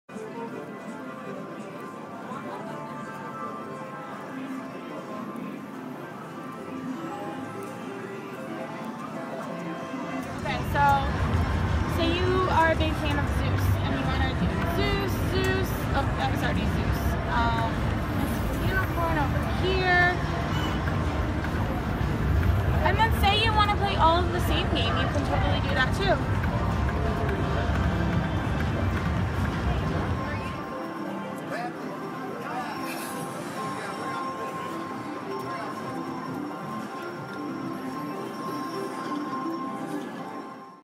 WMS Hot Hot 8
4 games at the same time not enough for you?  No worries, WMS Gaming's got your back.  At G2E 2014, WMS unveiled their new Hot Hot 8 slot which gives you the ability to play 8 classic games. Jungle Wild, Mystified Unicorn, Black Knight, Fallen Angels, Zeus, Great Zeus, Far East Fourtunes II and Raging Rhino can each be played at the same time or in whatever 8 game combinations you would like.  Check it out.  Thank you WMS for granting us the access to create this video